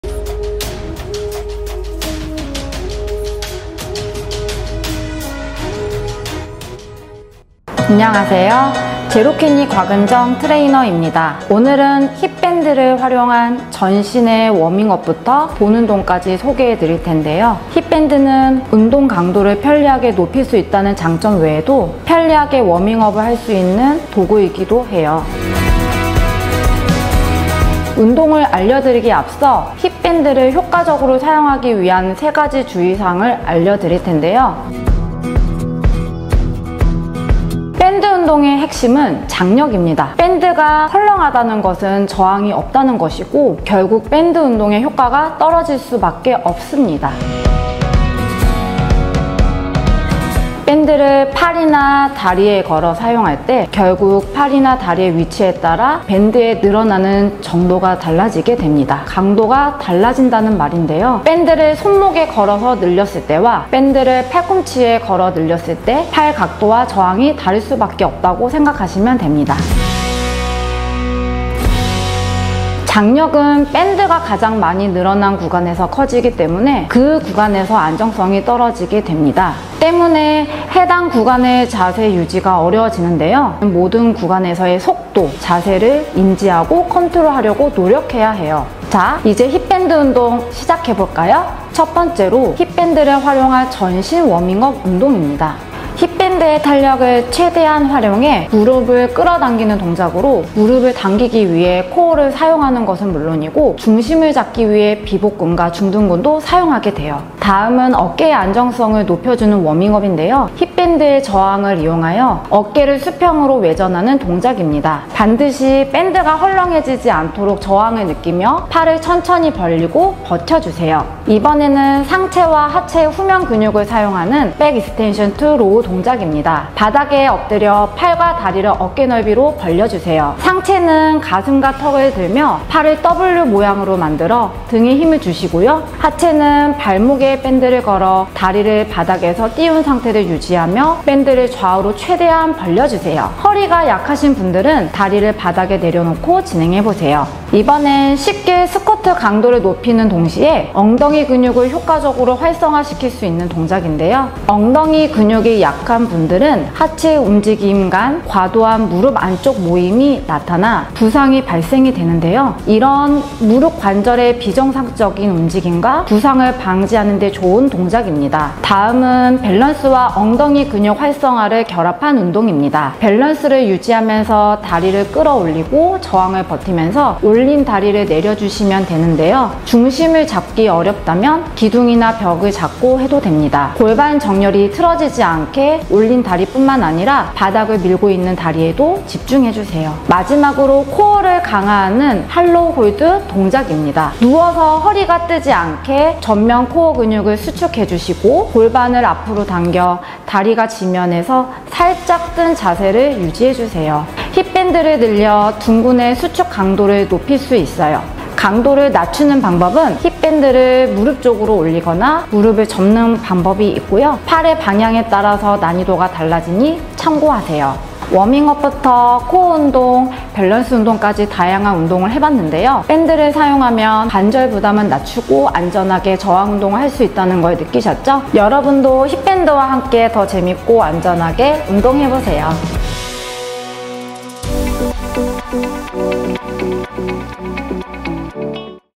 아직도 스쿼트만 한다고? 힙밴드 하나로 홈트 정복‼️
00:08 곽은정 트레이너 인사 및 운동 소개
00:26 3가지 주의사항
00:34 밴드의 장력 유지
00:45 밴드가 걸려있는 위치
01:09 올바른 자세 유지와 안정성
01:28 힙밴드 운동 6가지
01:30 스탠딩니업
01:46 어깨 수평 외전
02:00 백익스텐션 투 로우
02:26 스쿼트
02:47 싱글 레그 킥백
03:10 할로우 홀드
03:38 마무리 인사

오늘은 제로키니 곽은정 트레이너님과 함께 힙밴드를 활용한 전신 워밍업부터  본 운동을 소개합니다!
힙밴드로 운동할 경우 주의할 점 3가지와 힙밴드를 활용한 운동 6가지를 곽은정 트레이너님과 함께 해보아요!